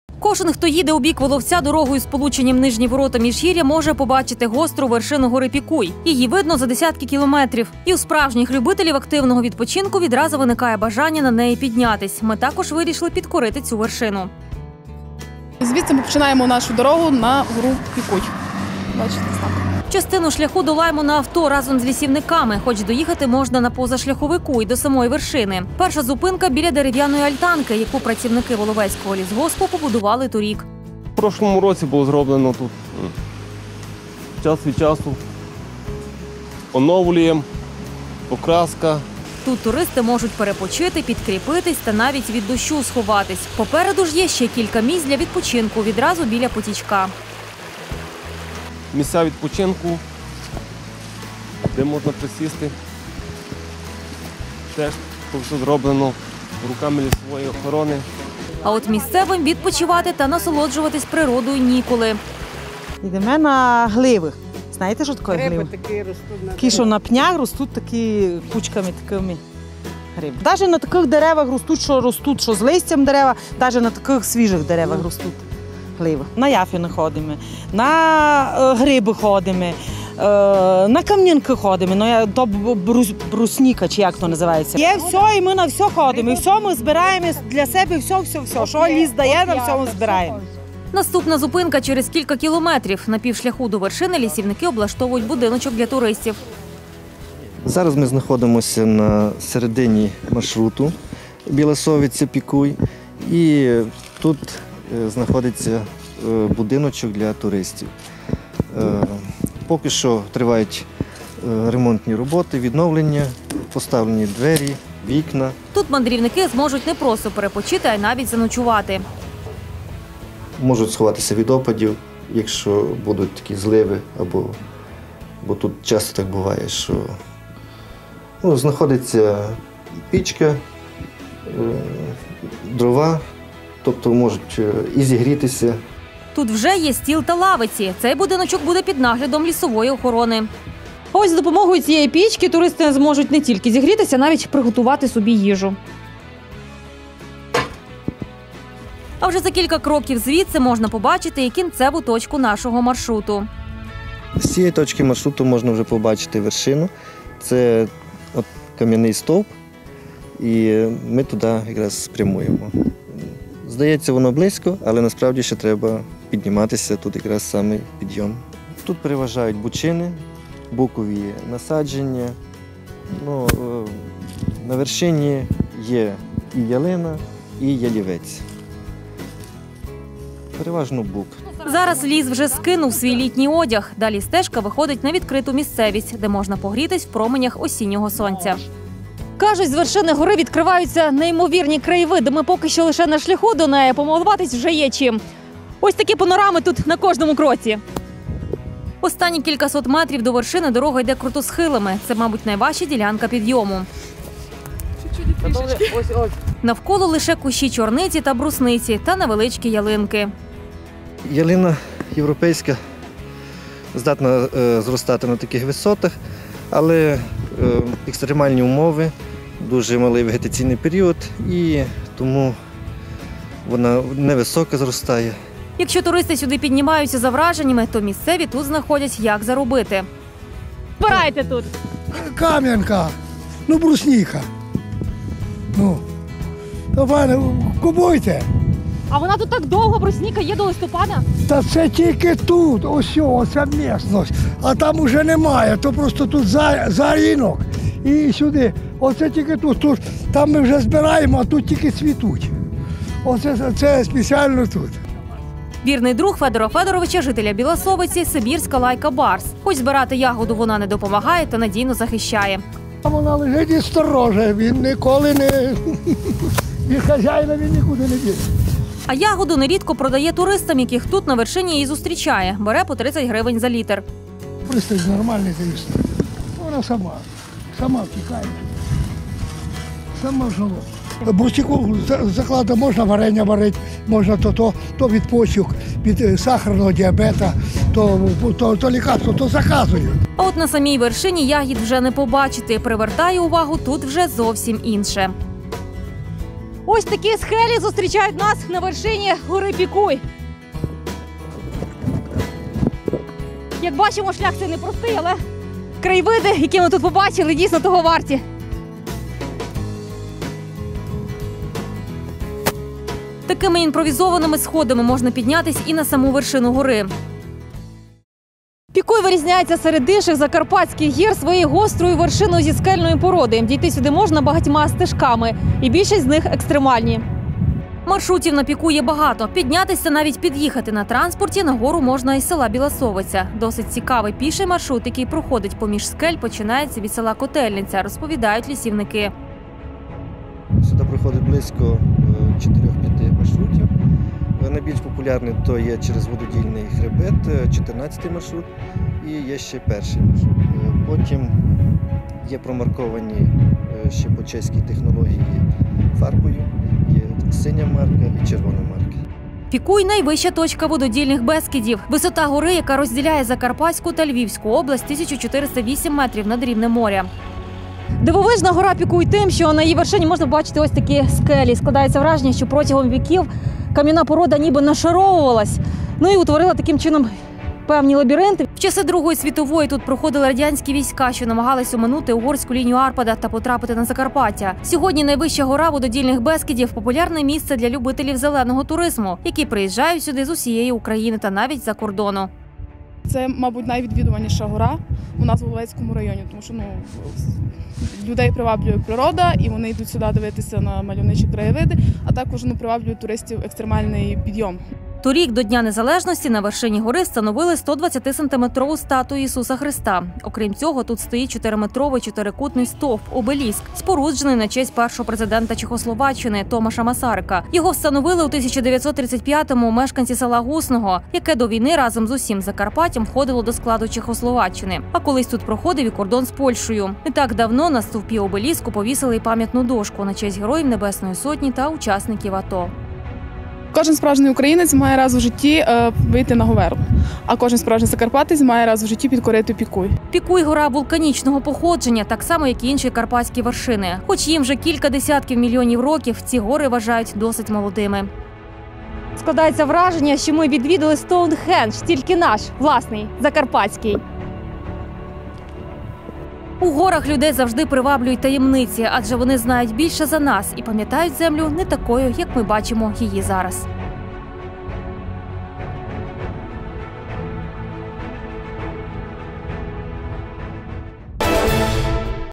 Найвища вершина Вододільного
Вона розділяє і об’єднує водночас. Гора Пікуй, найвища вершина Вододільного хребта, знаходиться на межі Львівської та Закарпатської областей. Щороку, аби побачити цю гору, сюди їдуть туристи з різних куточків України та  мандрівники з інших країн. Торік, до Дня Незалежності, на її вершині встановили 120-сантиметрову статую Ісуса Христа. Окрім цього тут стоїть чотириметровий стовп, який встановили  багато років тому на честь першого президента Чехословаччини Томаша Масарика, в подяку за розвиток верховинського краю.